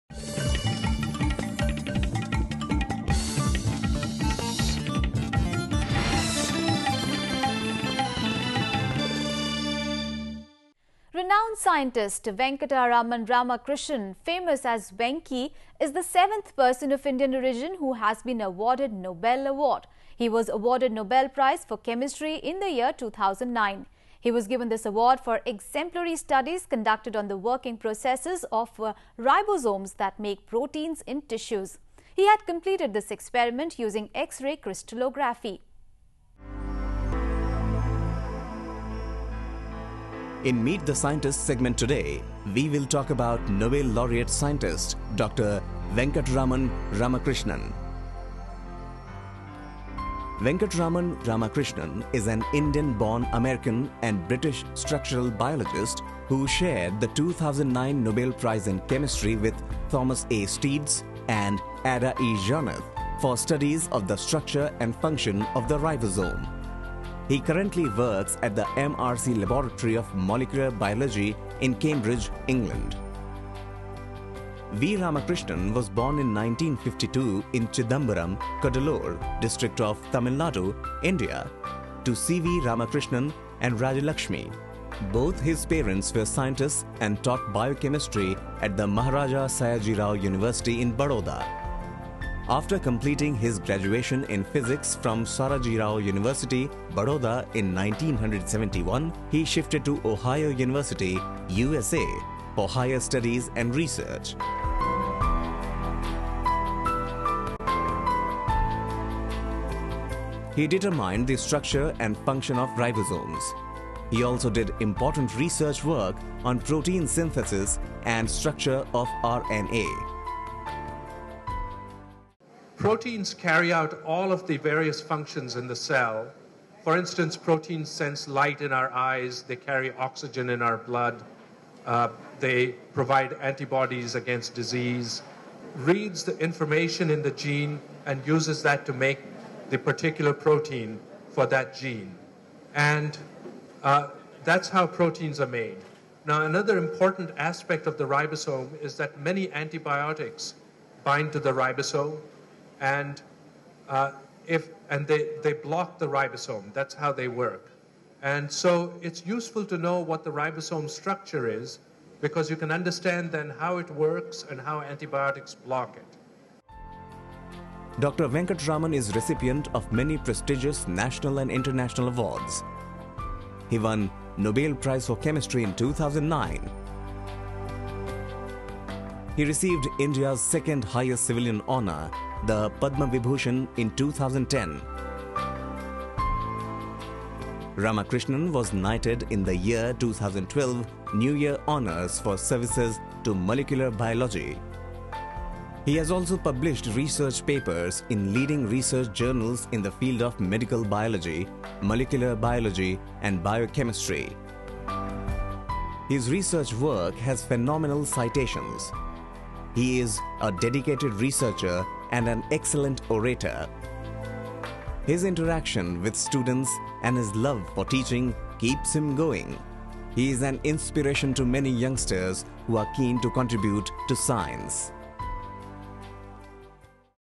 venkatraman Ramakrishnan.mp4
Venkatraman "Venki" Ramakrishnan, Kt, FRS is an Indian-born American and British structural biologist, who shared the 2009 Nobel Prize in Chemistry with Thomas A. Steitz and Ada E. Yonath, "for studies of the structure and function of the ribosome".